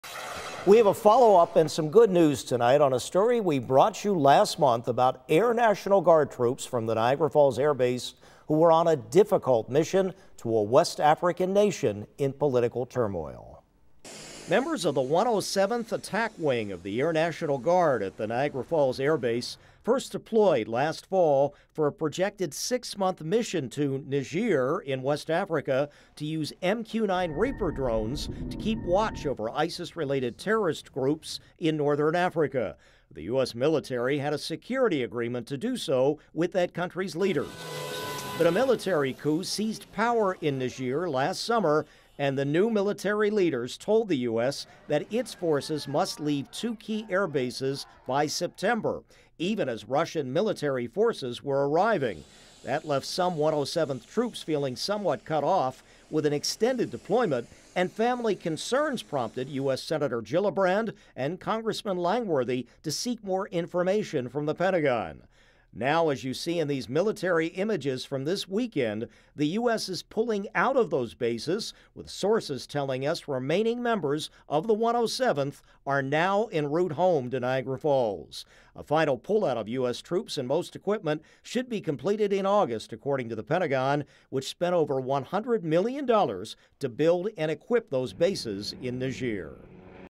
Niagara Falls airbase troops leave Niger; U.S. pulls out following military coup
A final pullout of U.S. troops and most equipment should be completed in August, according to the Pentagon.